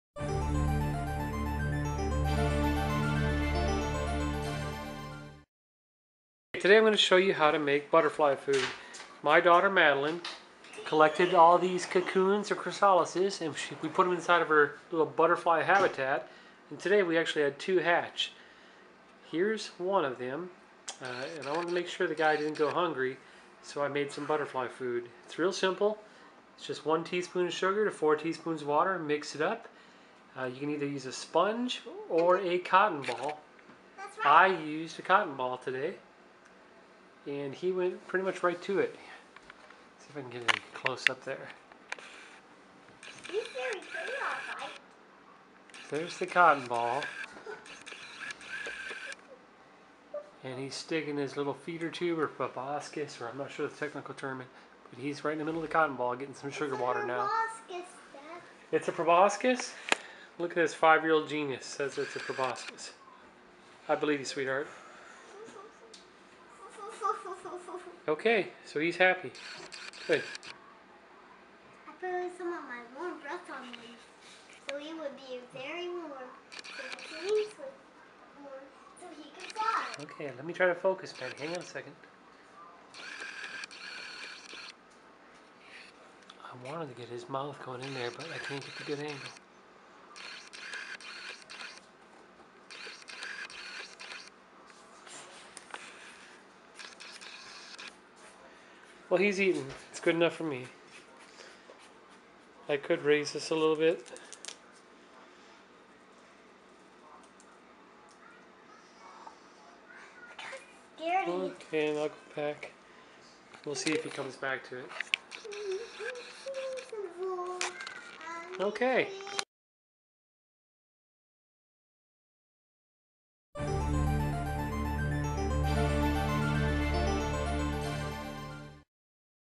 How To Make Butterfly Food
A step by step guide to making food for your butterfly.